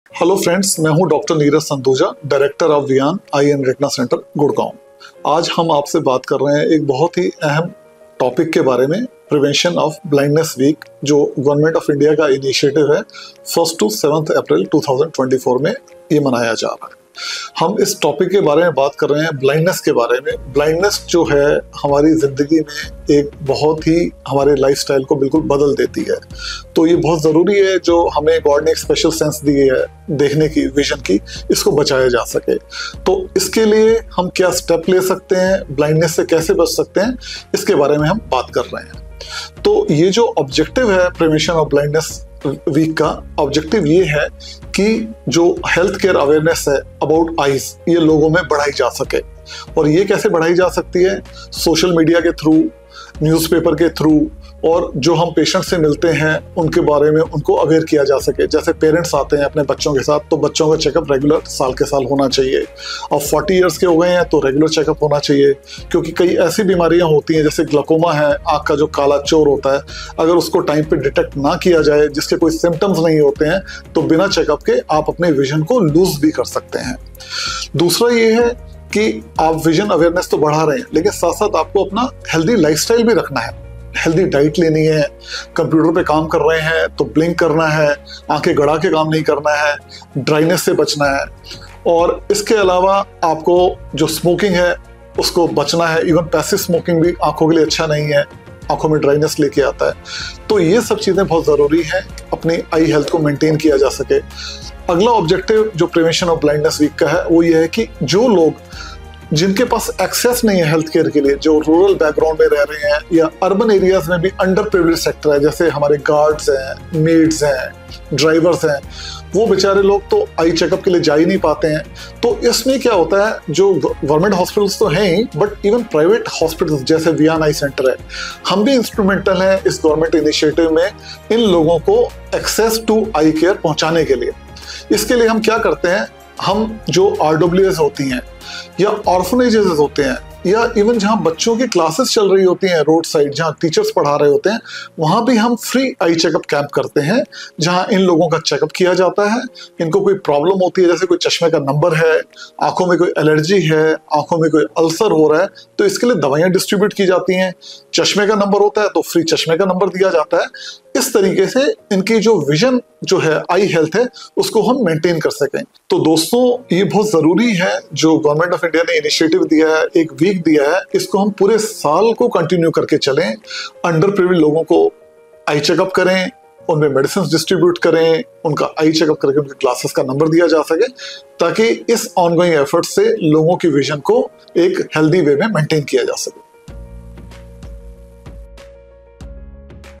Prevention Of Blindness Week | Dr. Neeraj Sanduja on Blindness | Viaan Eye & Retina Centre
Today we are going to talk about Prevention of Blindness Week, the objectives and the aim to celebrate this week. The major reasons why we observe 1st April to 7th April as Prevention of Blindness Week are explained here in this video by Dr. Neeraj Sanduja.
Let's hear him out!